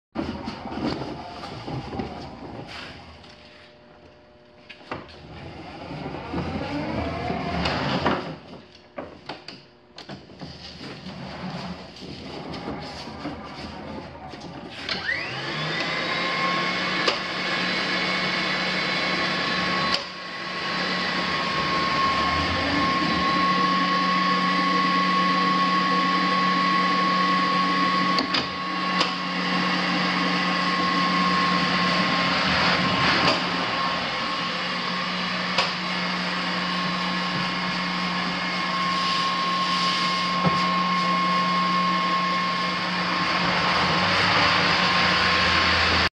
2010 Triple S Panther 26T floor scrubber for sale at auction | bidding closes January 26, 2022
2010 Triple S Panther 26T  floor scrubber
 Serial: 001089
 Watts: 1,200
 24VDC
 Amps: 50
It's located in Grinnell, IA, and we're selling it to the highest bidder regardless of price.